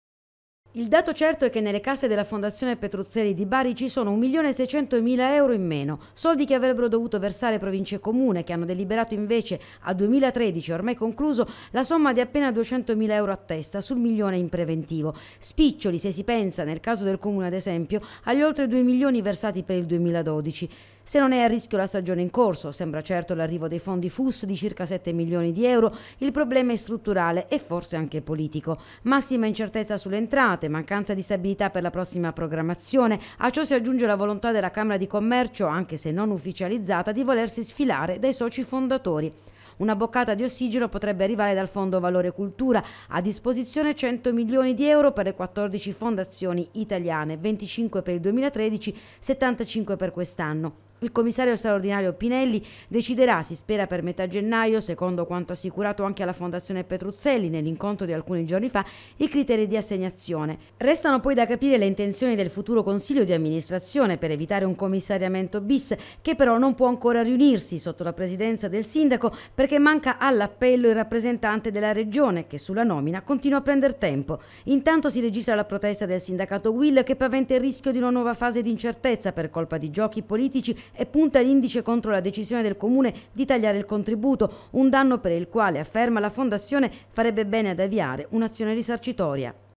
BARI PETRUZZELLI PROSPETTIVE
RESTA INCERTO IL FUTURO DELLA FONDAZIONE PETRUZZELLI DI BARI, ALLA LUCE DEI MANCATI O RISICATI VERSAMENTI DA PARTE DI COMUNE E PROVINCIA.